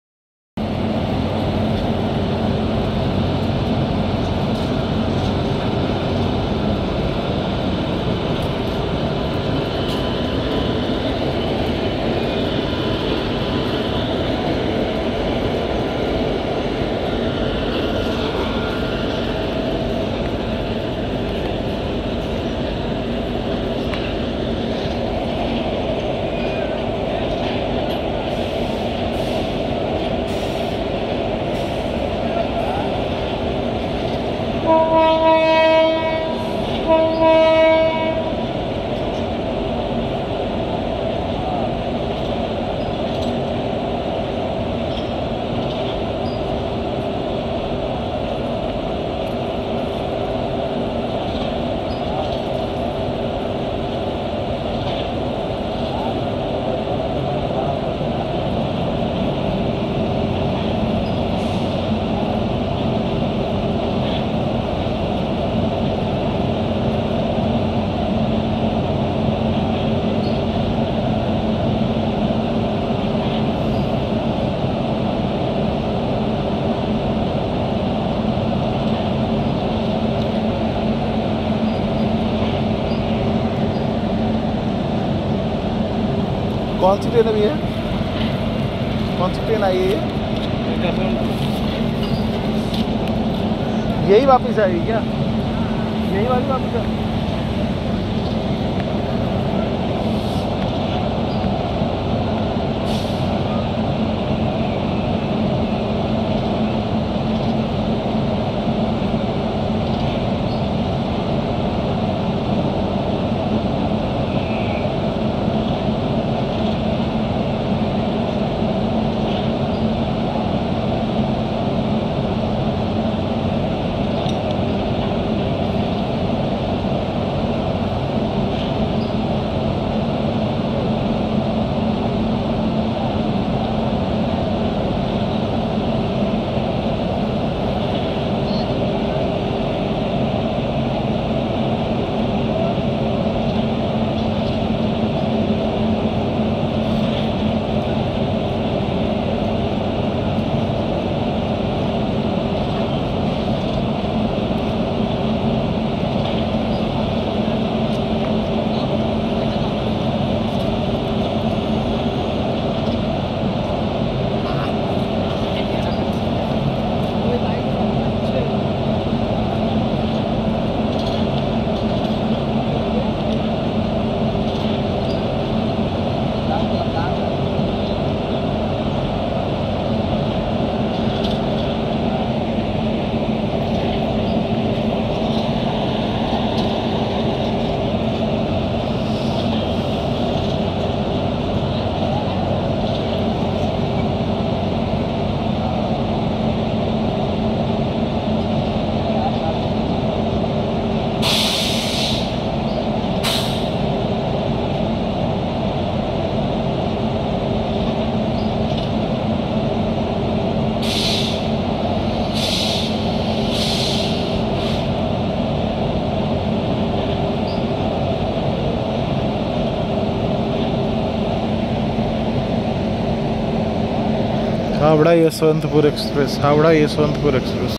5 hour delay Howrah - SMVT Bengaluru SF Express arrived at Howrah
Camera Used
Cannon SX70
Mobile Camera:

PBROS